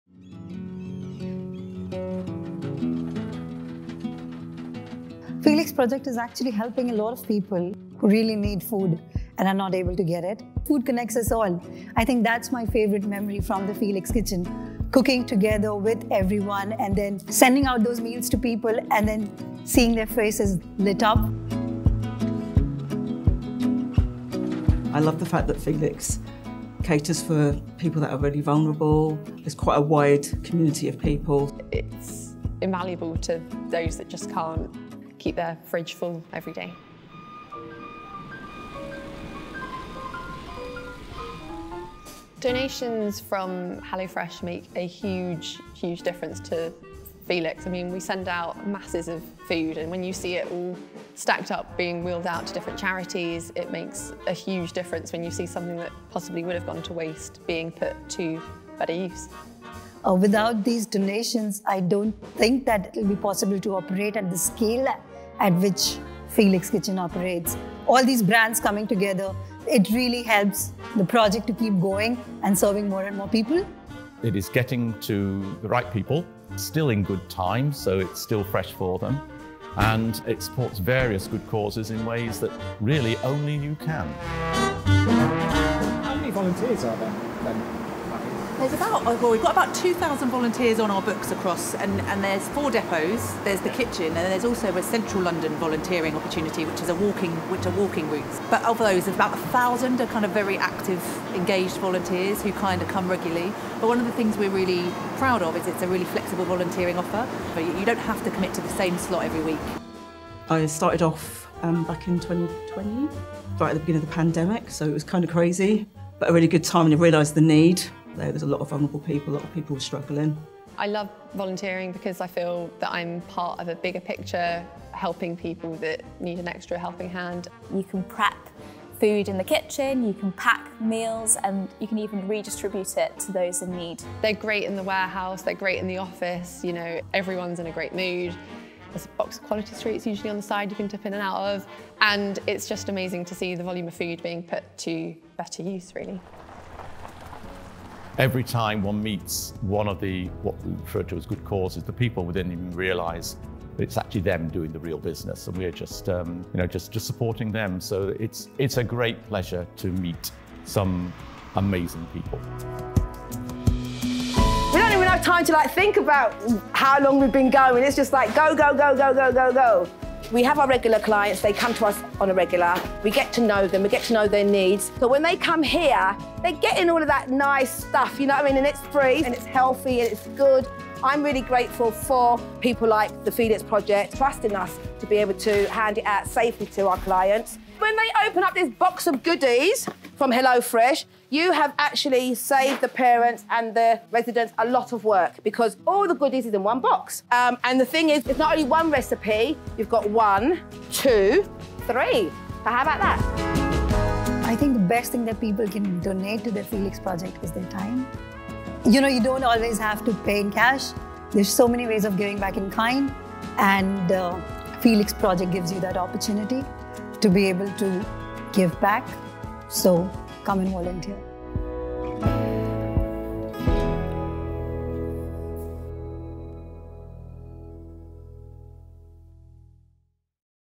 HelloFresh x The Felix Project
We're pleased to announce that HelloFresh UK has donated over 4.9 million meals to The Felix Project since our partnership began in 2017.

We have worked closely with The Felix Project to donate to thousands of people who are in need of food and look forward to continuing our partnership with the charity in the future.

You can also support this great cause through our Donate the Box feature across our website where you can donate your HelloFresh box to help feed those in need across the UK.